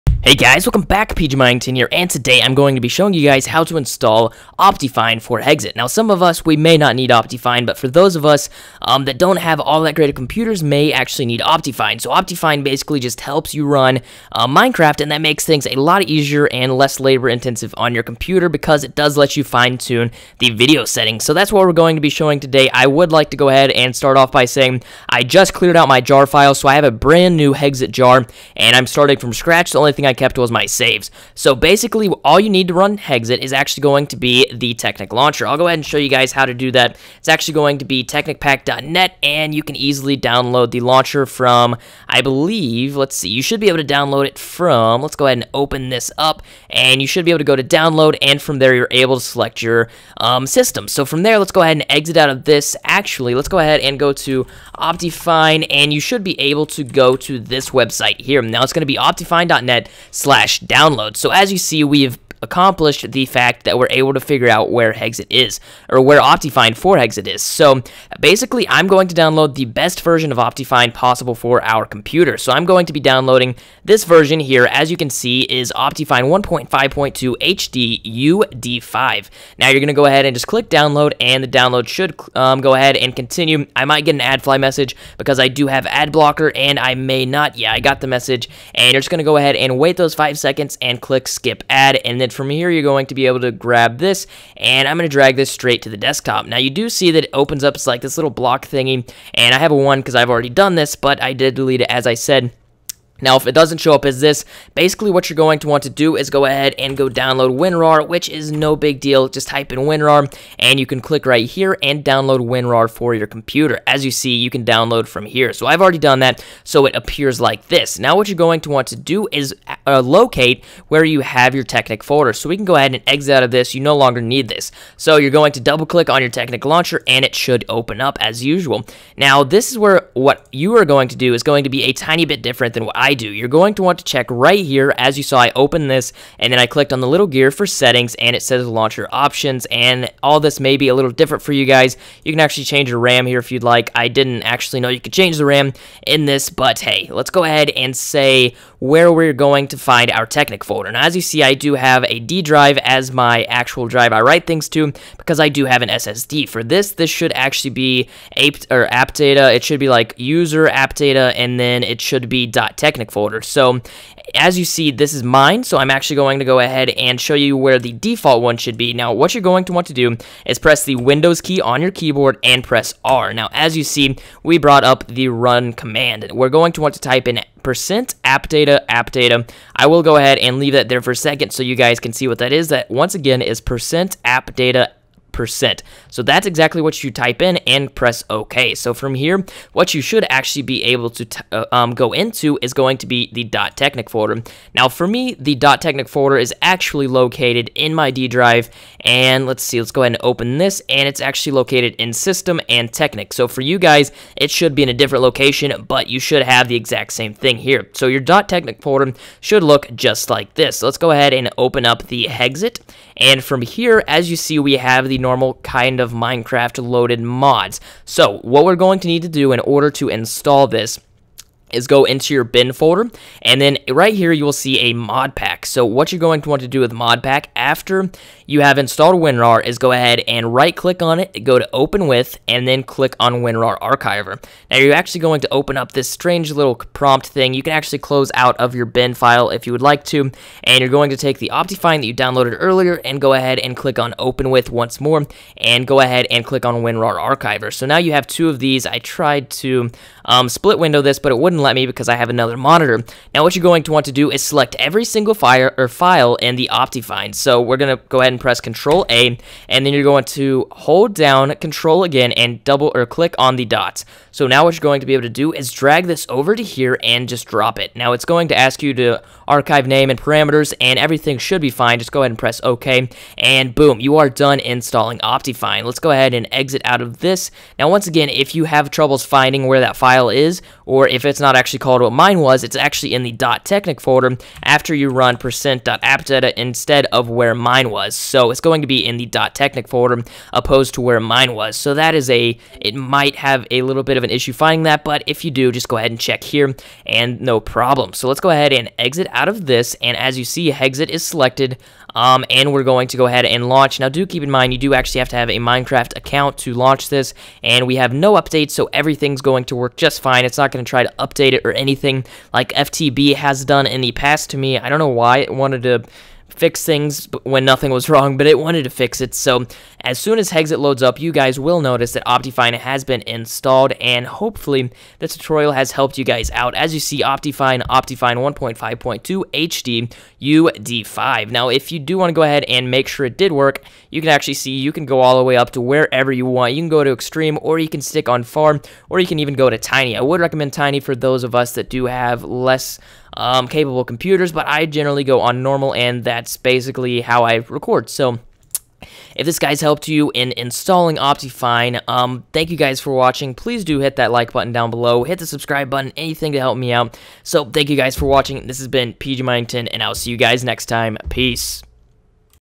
1.5.2 Hexxit Optifine Install
This tutorial shows how to install optifine for the hexxit mod pack!